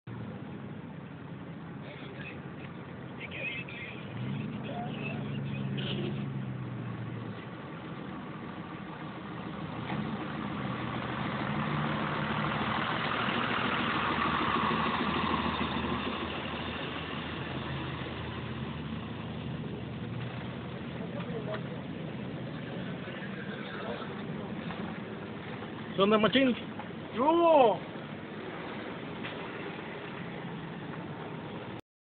GRUAS HUITRON 005
un pequeño choque de dos autobuse d frente, no calculo bien el compa